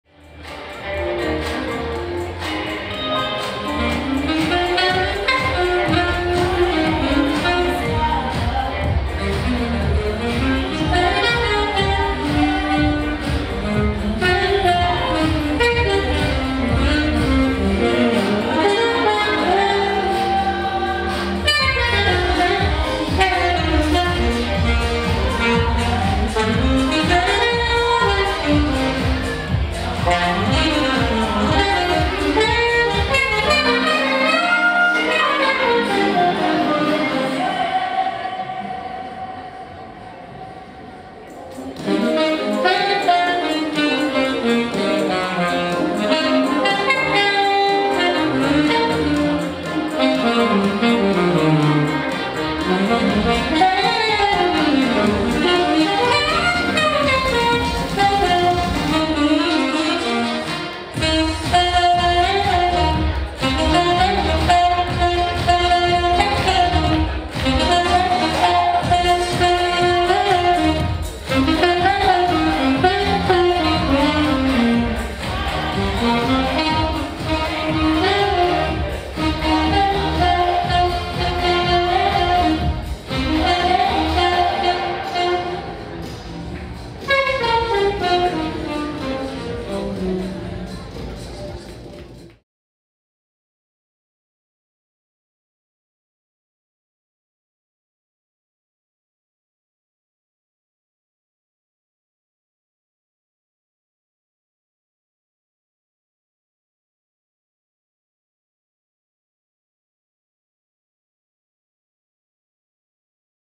REF: 135 - JAZZ / BOSSA SAXOPHONIST
Fabuloso saxofonista y flautista (travesera) de los estados unidos, con un toque muy moderno y preciso. Perfecto para amenizar un aperitivo y empezar a animar al público para la fiesta, también se puede adaptar para ofrecer un producto más tranquilo y sutil de background music. Estilos de bebop neoyorkino a west coast cool jazz y bossa.

 Fabulous saxophonist and flutist (transverse) from the United States, with a very modern and precise touch. Perfect for livening up an aperitif and starting to get the crowd excited for the party, it can also be adapted to offer a calmer and more subtle background music product. Styles from New York bebop to West Coast cool jazz and bossa.